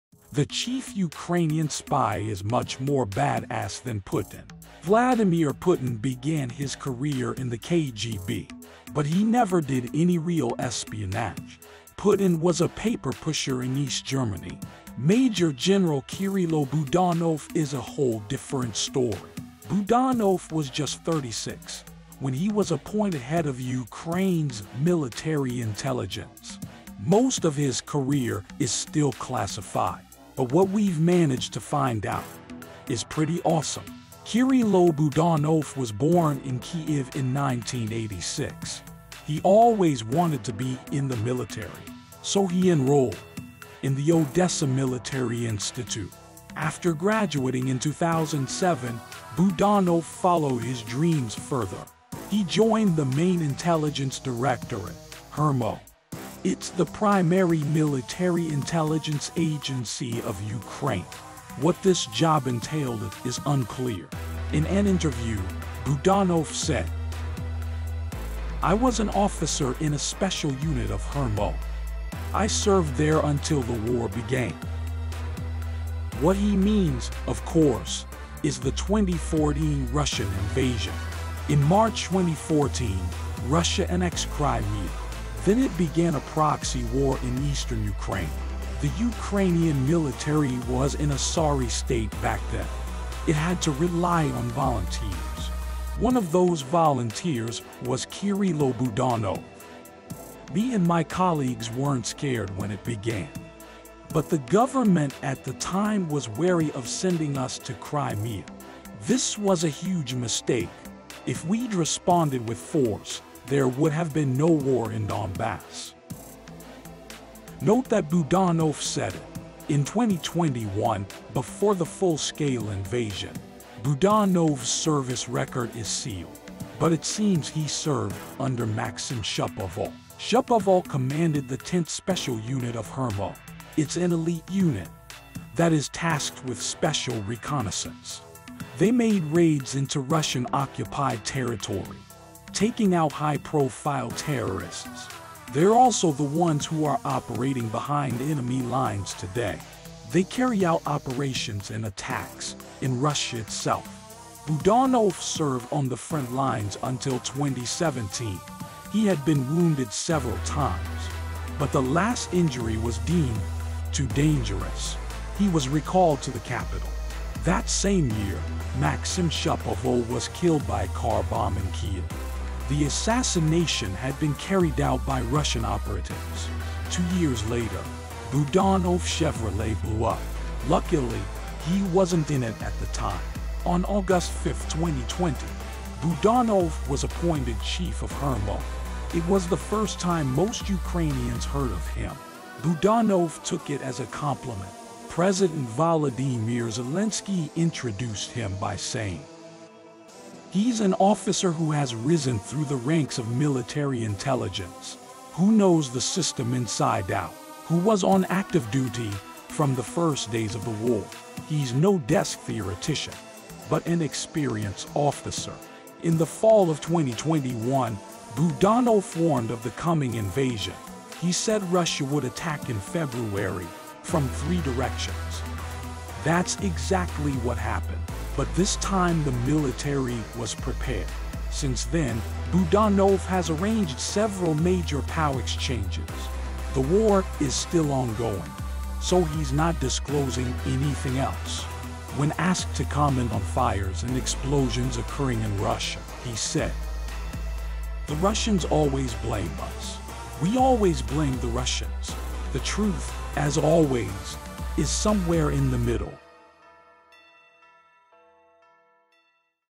The Chief Ukrainian Spy Is Much More Badass Than Putin
38-year-old Major General Kyrylo Budanov prefers to operate in the shadows.